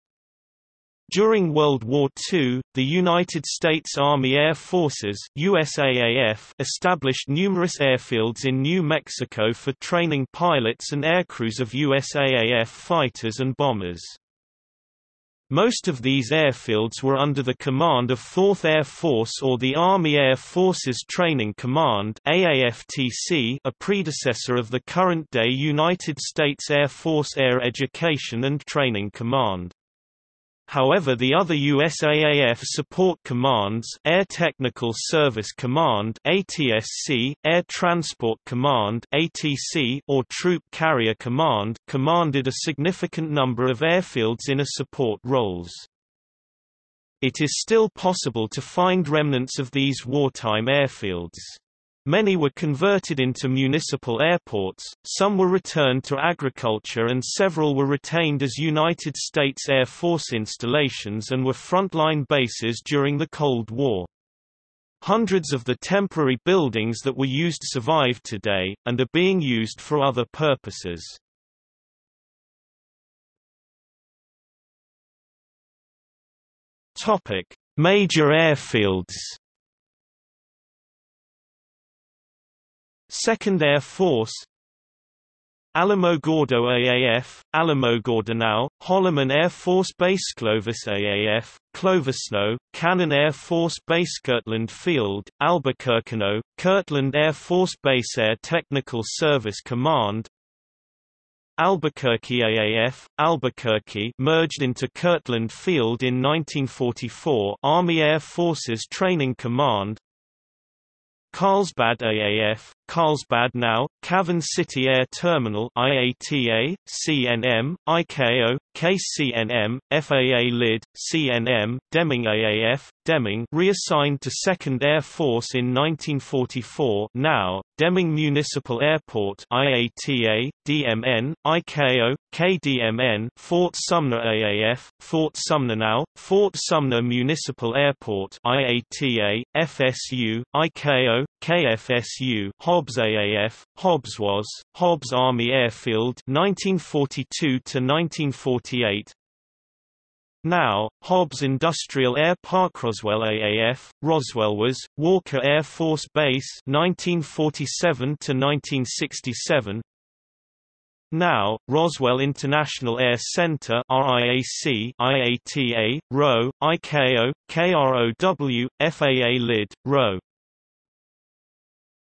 New Mexico World War II Army Airfields | Wikipedia audio article
00:01:13 Major Airfields

- Socrates

SUMMARY
During World War II, the United States Army Air Forces (USAAF) established numerous airfields in New Mexico for training pilots and aircrews of USAAF fighters and bombers.
Most of these airfields were under the command of Fourth Air Force or the Army Air Forces Training Command (AAFTC) (A predecessor of the current-day United States Air Force Air Education and Training Command).  However the other USAAF support commands (Air Technical Service Command (ATSC); Air Transport Command (ATC) or Troop Carrier Command) commanded a significant number of airfields in a support roles.
It is still possible to find remnants of these wartime airfields. Many were converted into municipal airports, some were returned to agriculture and several were retained as United States Air Force installations and were front-line bases during the Cold War. Hundreds of the temporary buildings that were used survive today, and are being used for other purposes.